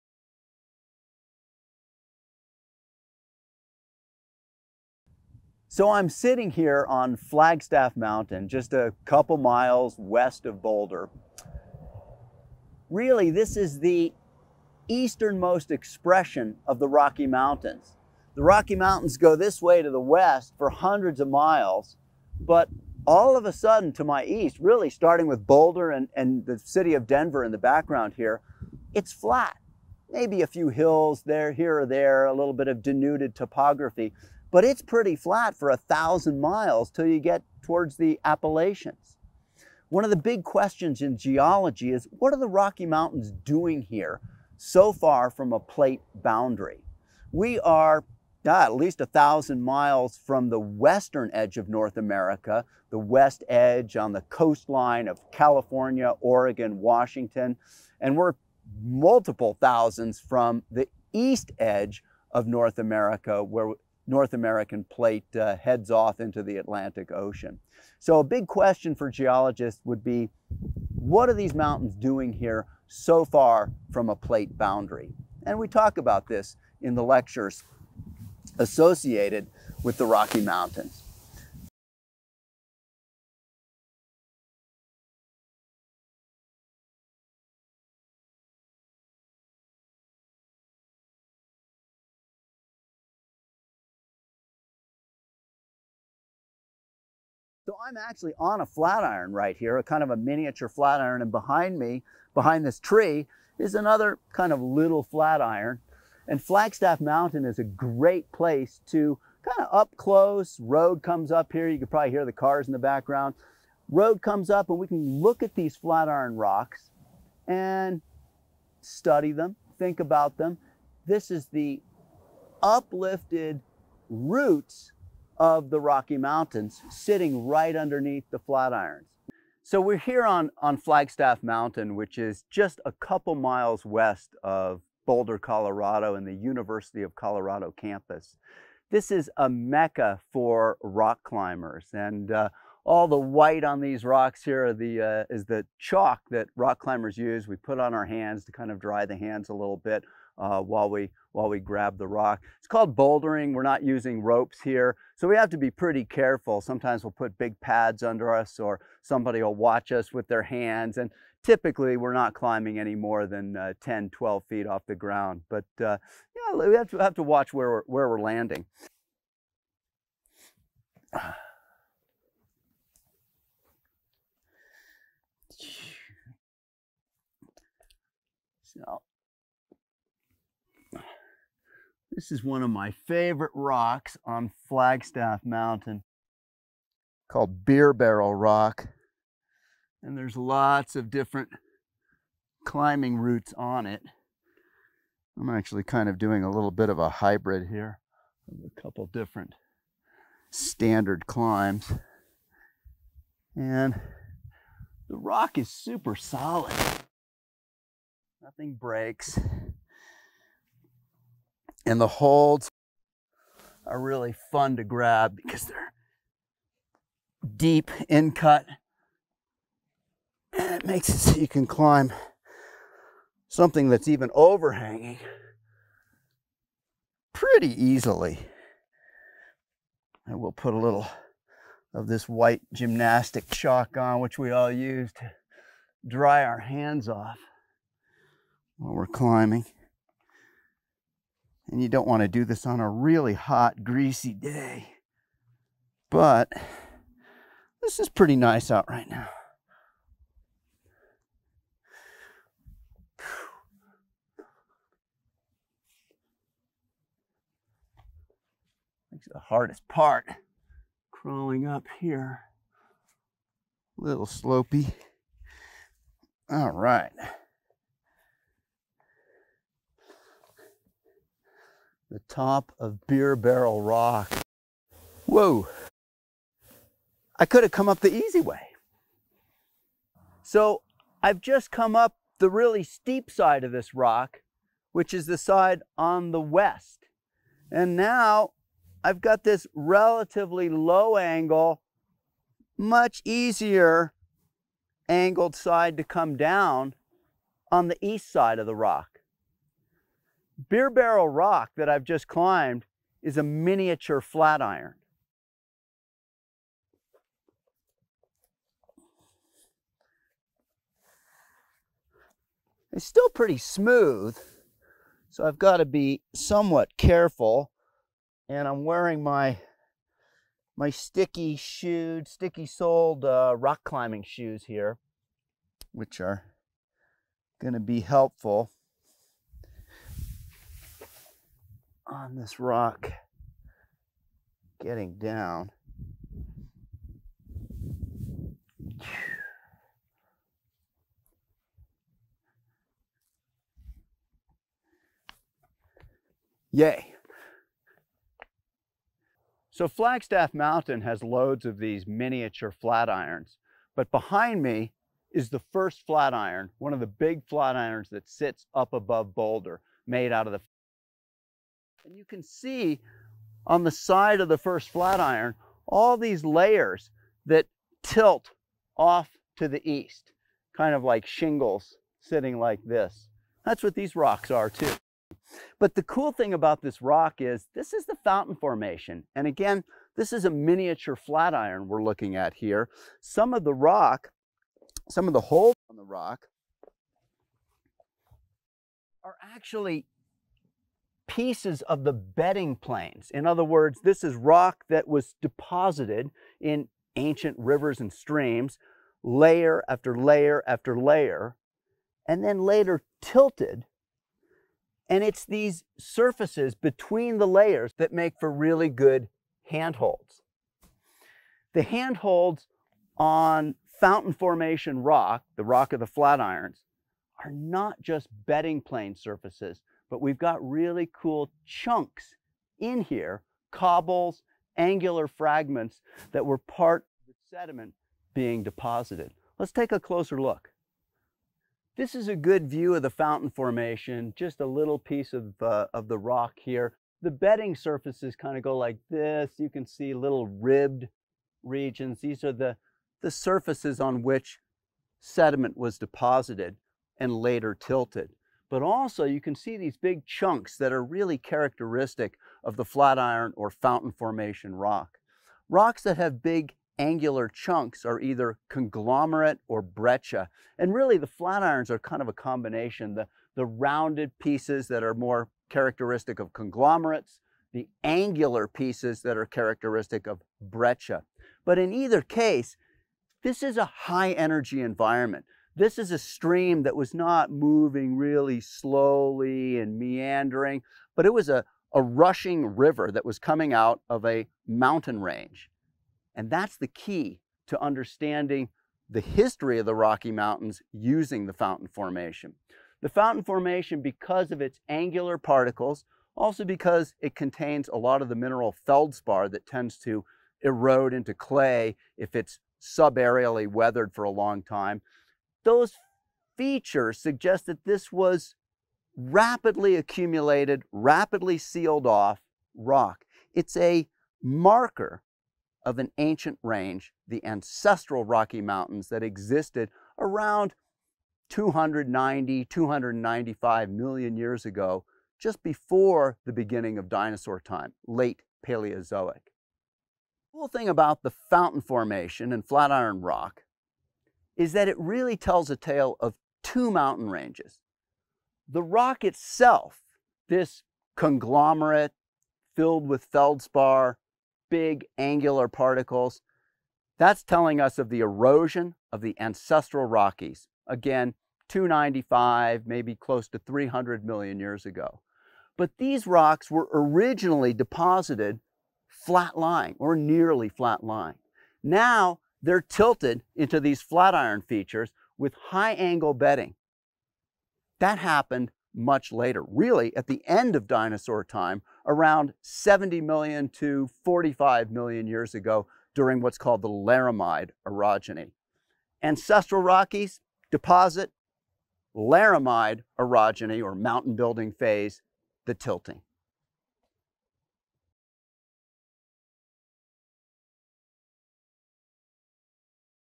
Flagstaff Mountain Field Trip Video Example
Deep Time: Discovering an Ancient Earth and the History of the Rocky Mountains with Alan Lester
Lecture title: Flagstaff Mountain: The Fountain Formation and the Ancestral Rockies
Coursera Link: